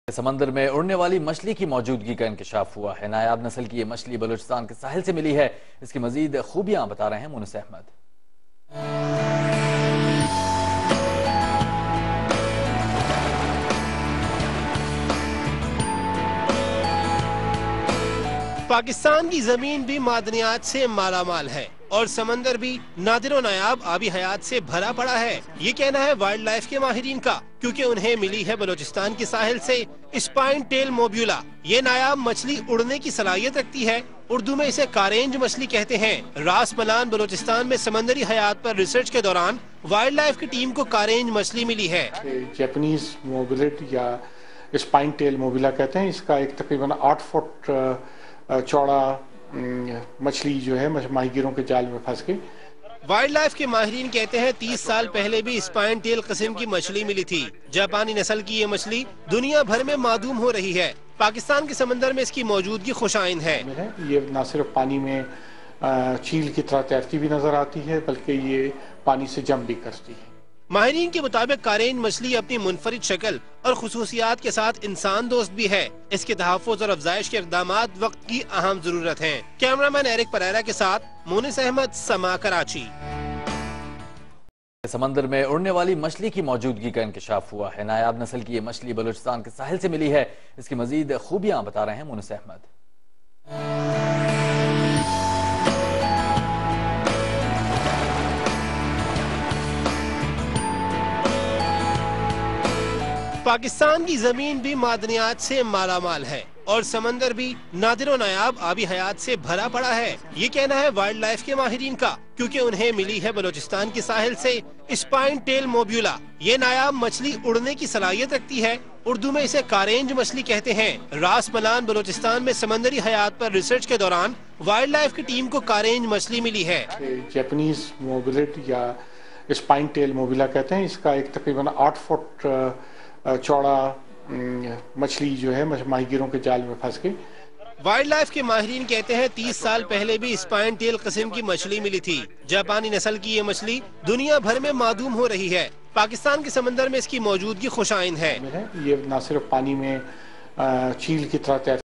Flying Fish In Pakistan
Wild Life Researchers Have Found Spinetail Mobula (Flying Fish) From Balochistan Sea.

A RARE SPECIES OF FLYING FISH THE Spinetail Mobula FOUND IN BALUCHISTAN,PAKISTAN COASTAL SEA.

PIA Pakistan International Airlines B777-200/ER flight PK788 London to Karachi from the flight deck.

Air Swimmer flying fish. Check out more tech news over at